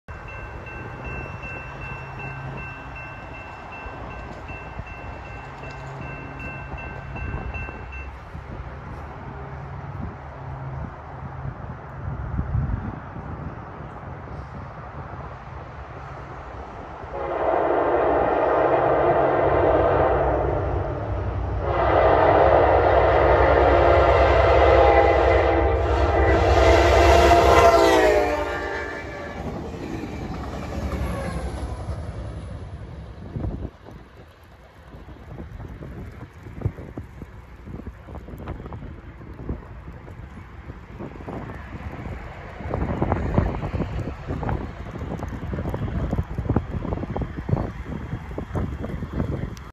Amtrak Train 73 Flies Through Harrisburg NC with Amtrak leader 1869 (11-3-2021)
Amtrak Piedmont Train 73 flies past CP Shamrock in Harrisburg North Carolina with Amtrak F59 1869 leading.